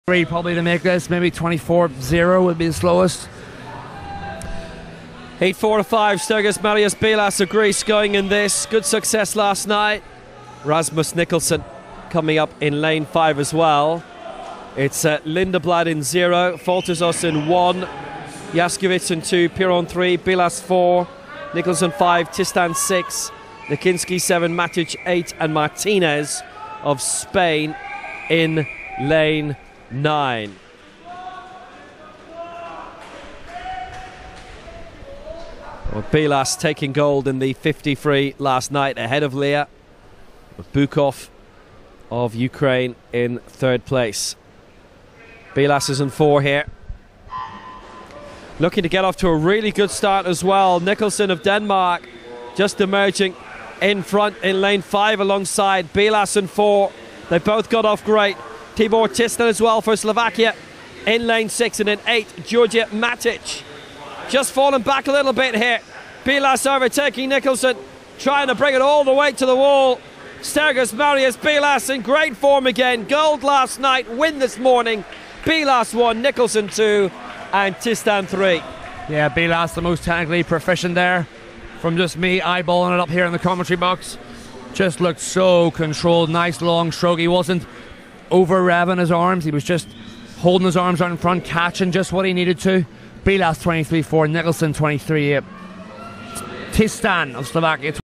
ΜΠΙΛΑΣ ΒΟΛΤΕΖΟΣ 50Μ ΠΕΤΑΛΟΥΔΑ ΠΡΟΚΡΙΜΑΤΙΚΑ
LEN Ευρωπαϊκό Πρωτάθλημα Κολύμβησης U23 - ΔΟΥΒΛΙΝΟ 11-13 Αυγούστου 2023